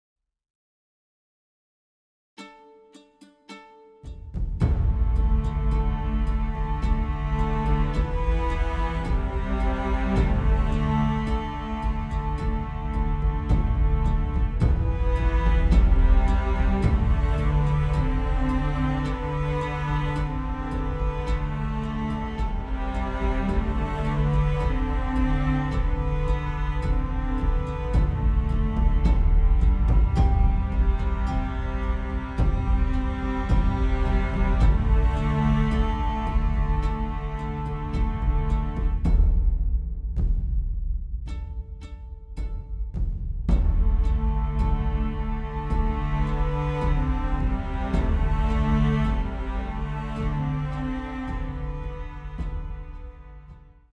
St. John's Bible Exhibit Opening Night, Canton Museum
Illuminating the Word: The St. John's Bible.  Opening night at the Canton Museum of Art, December 5, 2013